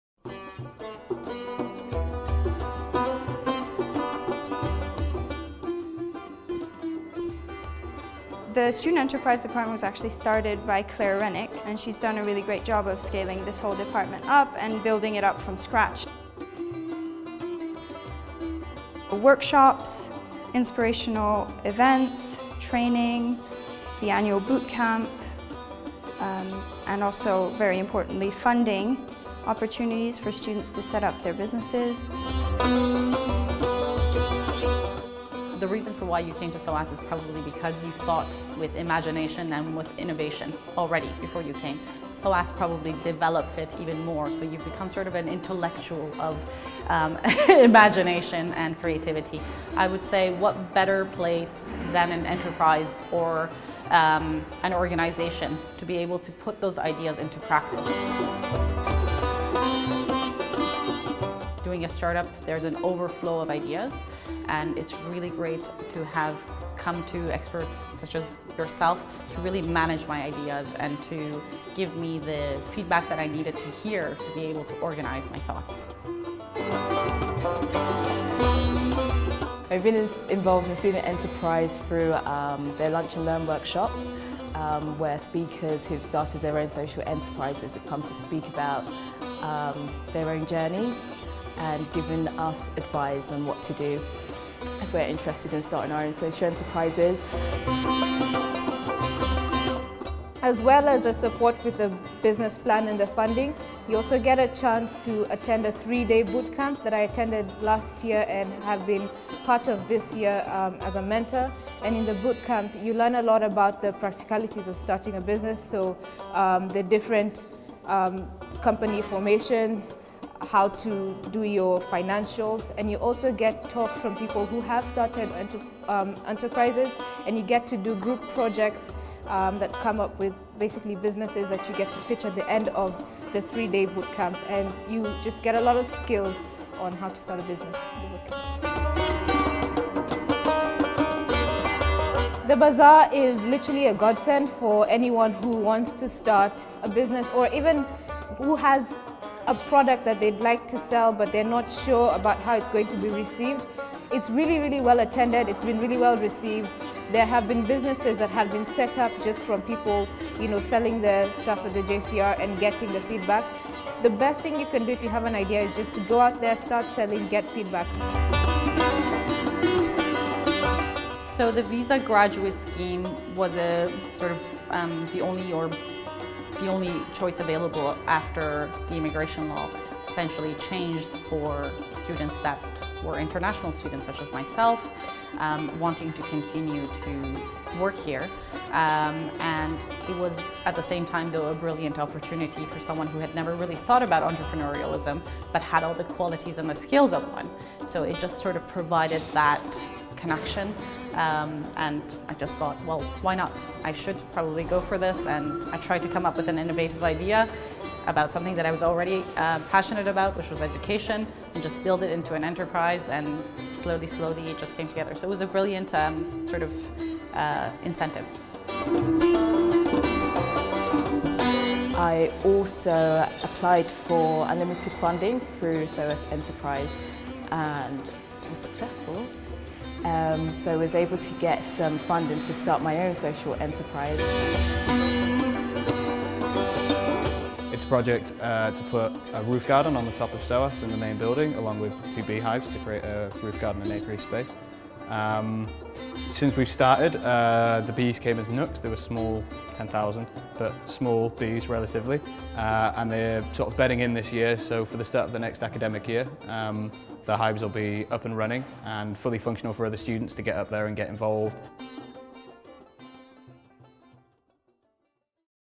From inspiration to action - SOAS Student Enterprise at SOAS
From boot camps to the annual Bazaar; and the Graduate Entrepreneur Visa Scheme to the allocation of funding for start up projects - the SOAS Student Enterprise team is here to help.